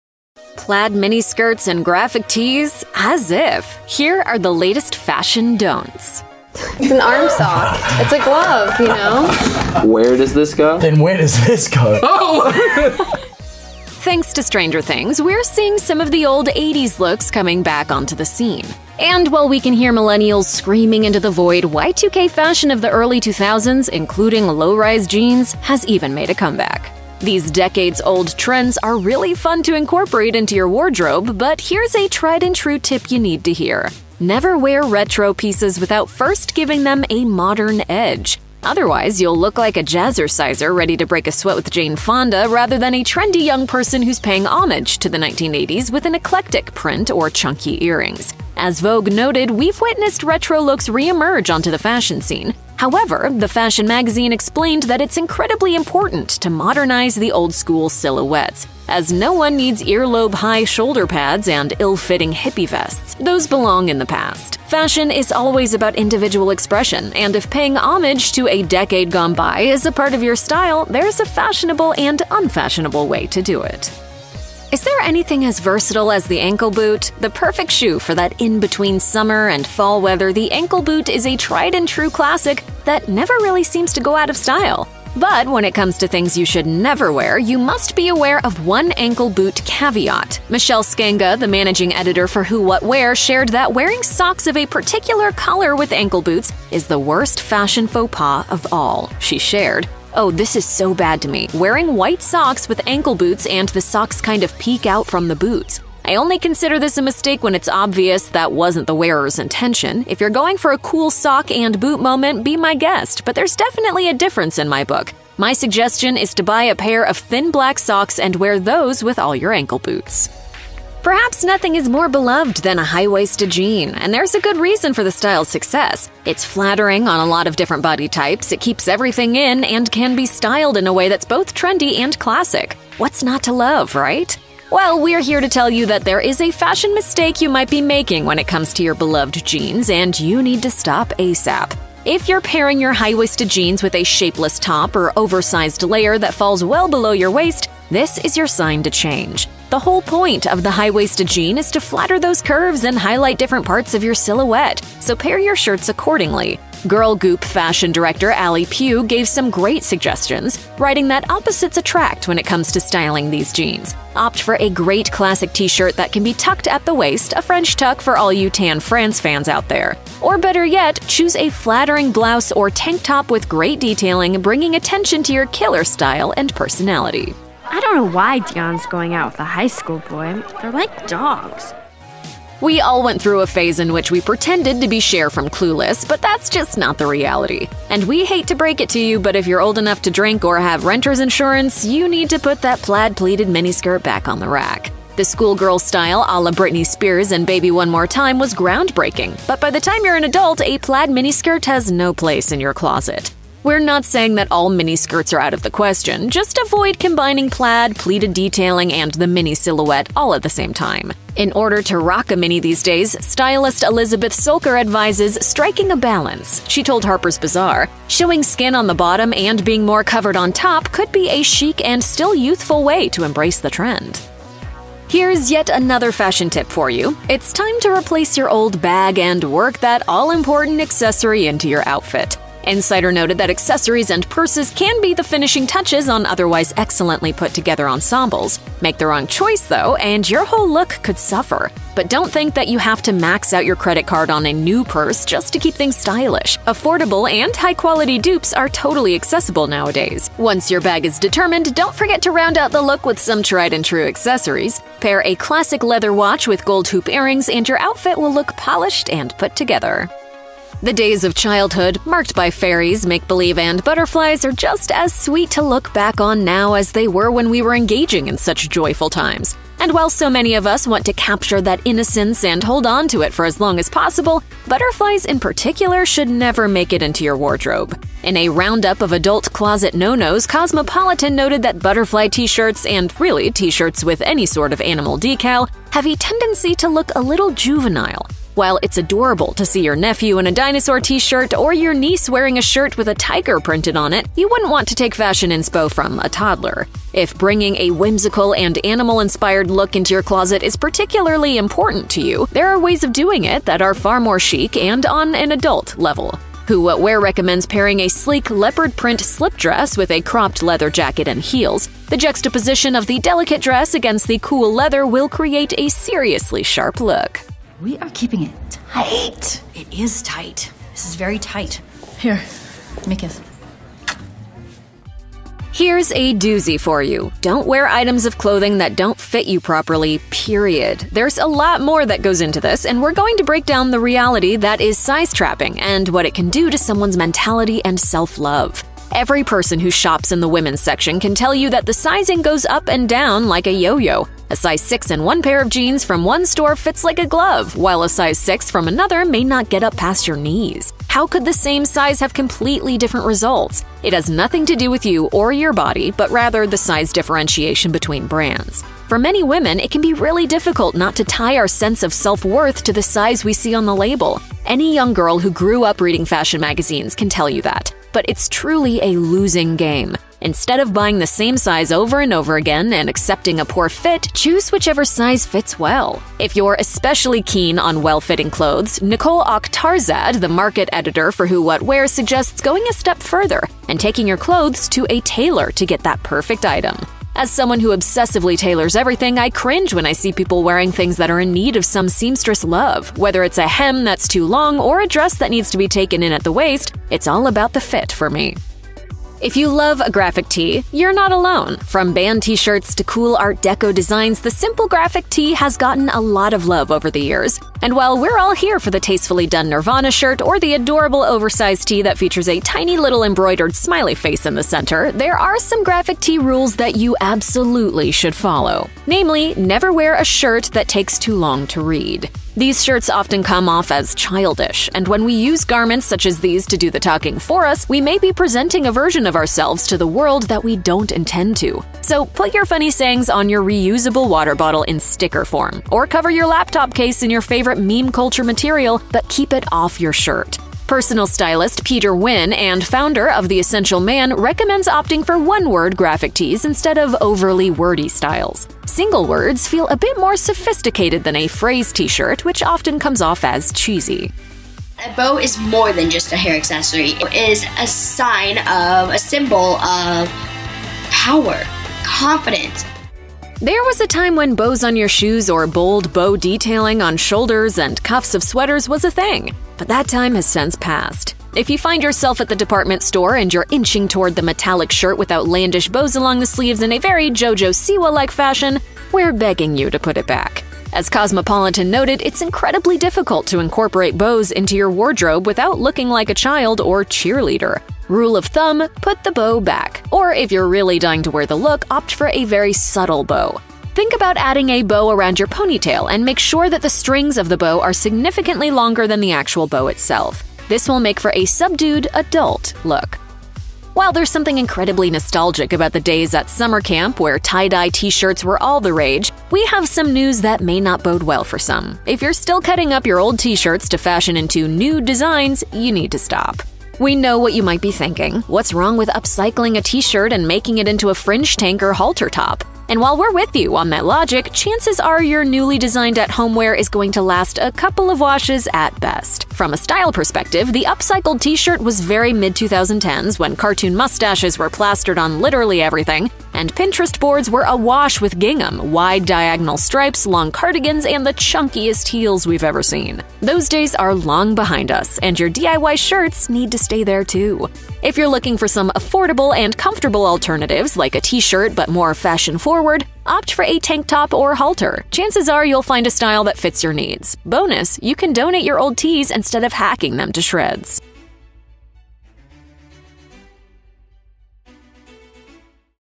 Huge Fashion Mistakes You Didn't Know You Were Making
The cardinal sin of footwear? The cute accessory that turned cringey craze? Drop the scissors and tie-dye — that look isn’t as good as you think it is.

Going retro | 0:00
Ankle boot no-no's | 1:14
High-waisted jeans | 2:00
The plaid mini skirt | 2:57
Accessories | 3:44
Bye to butterflies | 4:20
Clothes that don't fit | 5:17
Graphic tees | 6:39
Bows | 7:34
DIY Shirt Hacks | 8:23